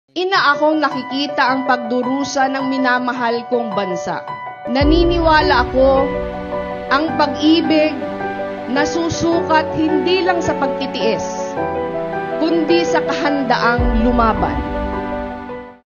Reminiscing Leni Robredo's 2022 Campaign Efforts | Pink Wave Campaign Trailer
Join us as we take a trip down memory lane
and revisit Leni Robredo's past campaign during the 2022 elections. Despite not winning the campaign, we will highlight and celebrate her tireless efforts in the Pink Wave Campaign.